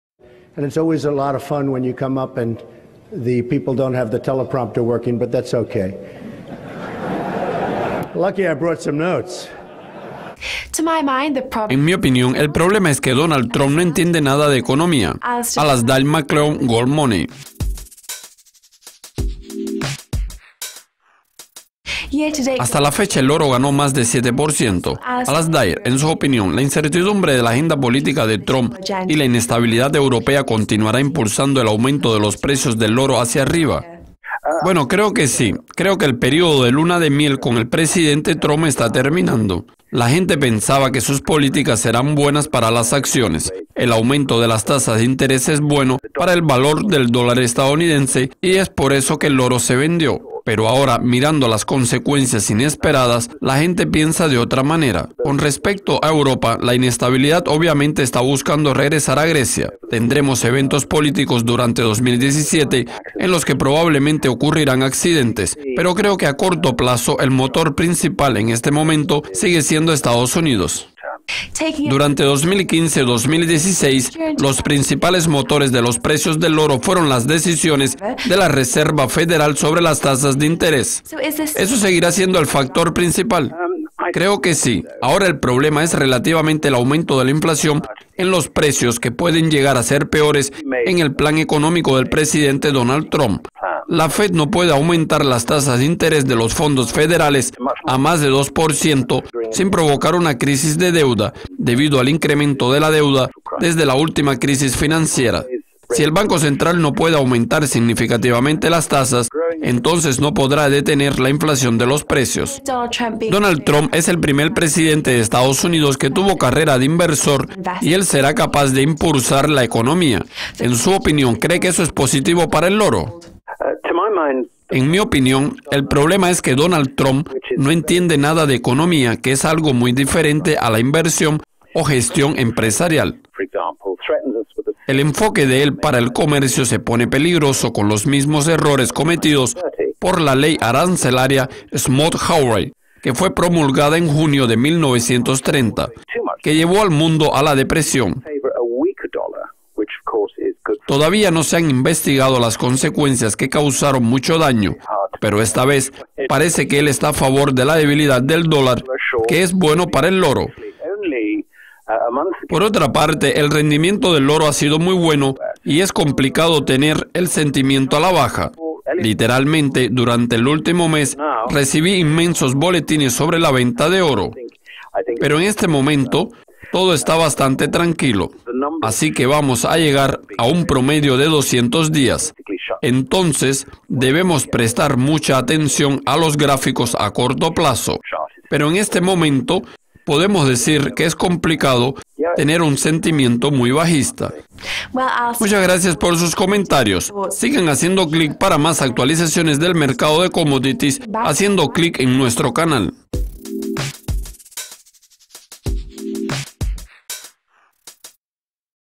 USD débil es bueno para oro
¿Los principales motores de oro han cambiado desde que Donald Trump ganó la presidencia? Alasdair Macleod,Gold Money